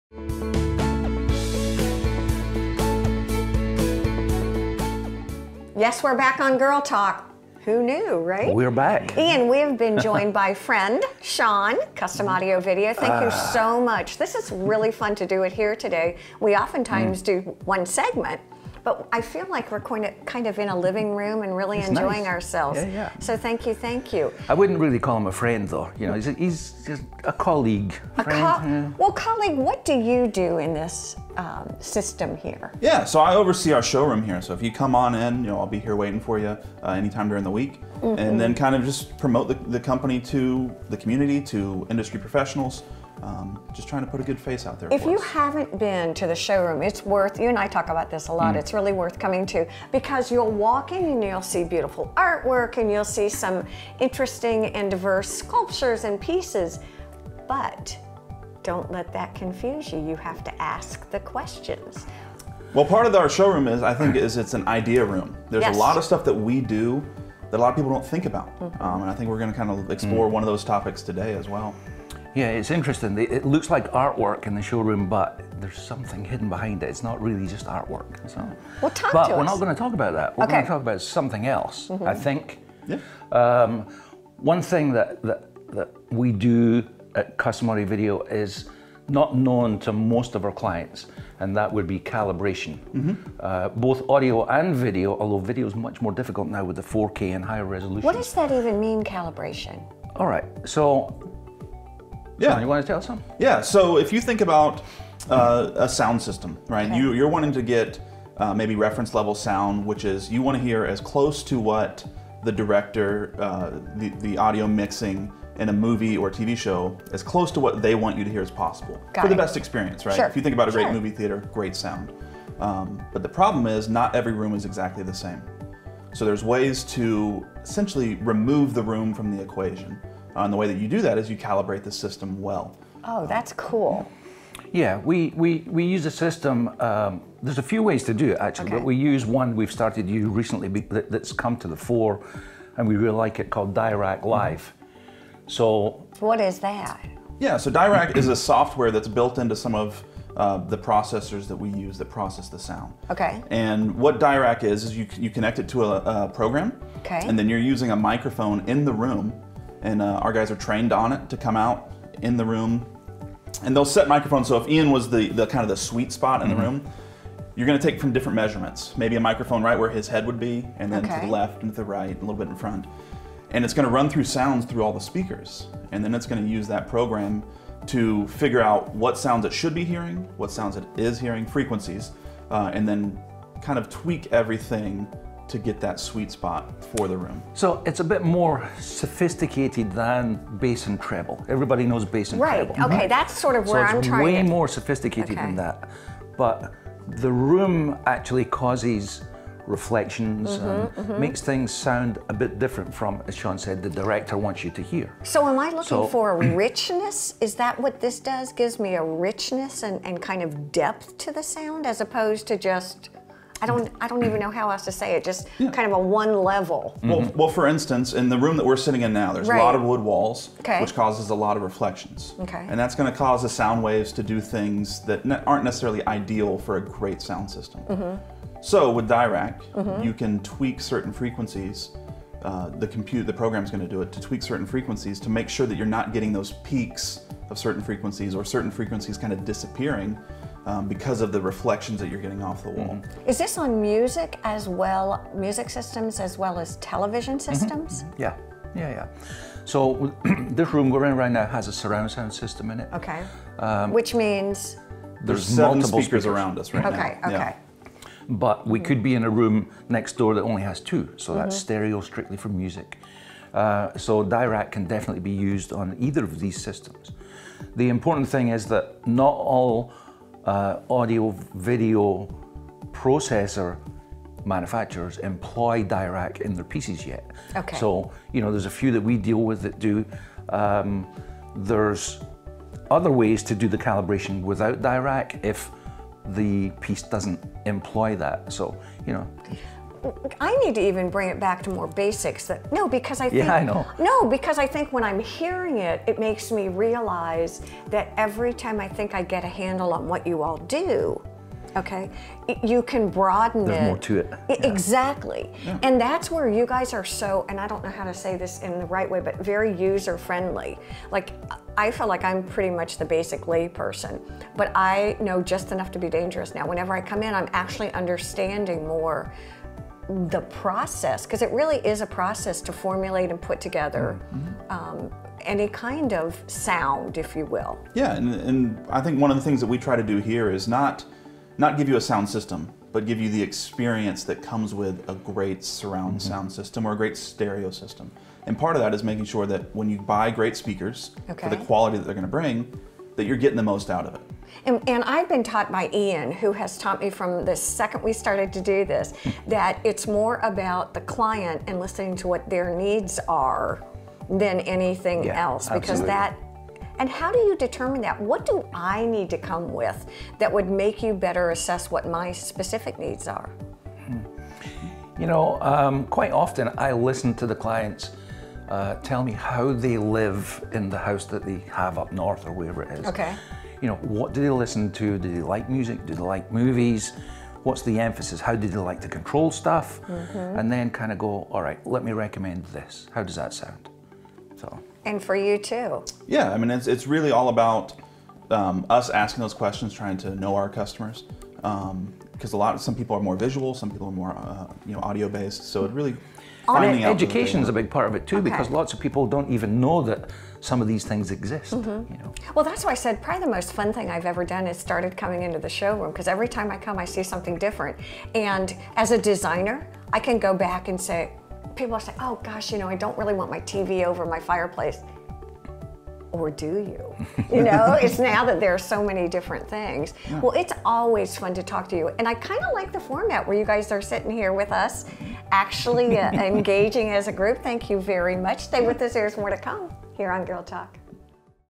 Girl Talk November 2019 Part 2 of 3
In this segment Debi, Iain, and Sean discuss audio calibration and the processes that go into creating a great sound system. Assess a clients needs and the room a system will be installed in are crucial to getting the best quality and value out of an audio system, whether a small stereo or grand surround sound system.